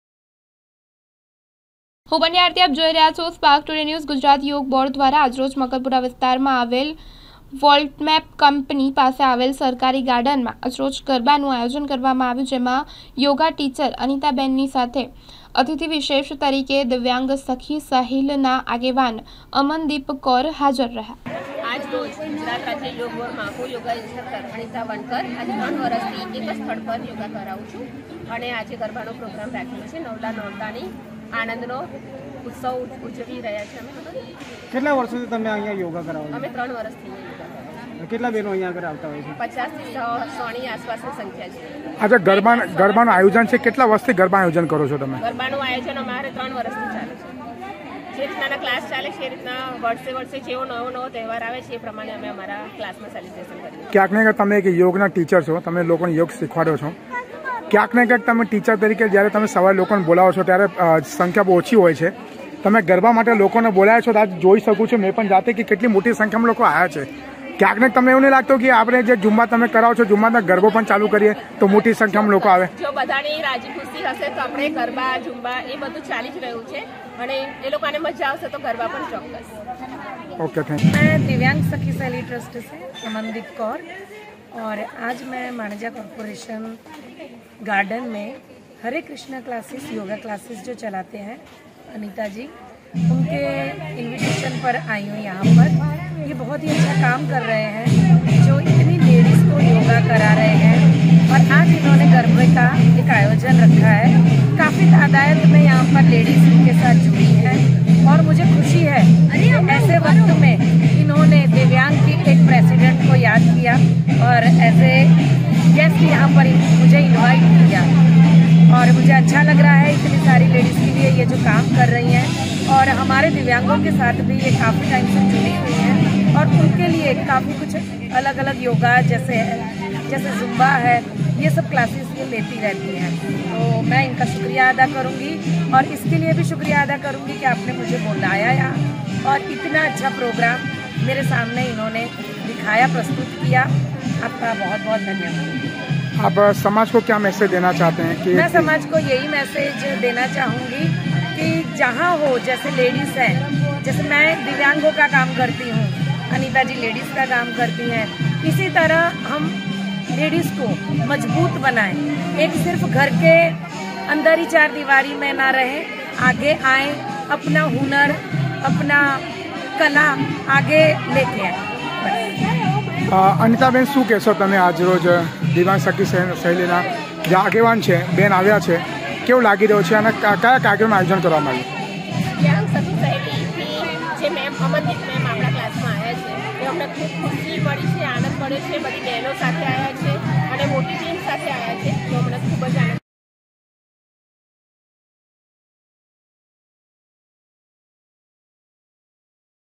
ગુજરાત યોગ બોર્ડ દ્વારા મકરપુરા વિસ્તારમાં ગરબા આયોજન 2023 | Spark Today News
ગુજરાત યોગ બોર્ડ દ્વારા મકરપુરા વિસ્તારમાં ગરબા આયોજન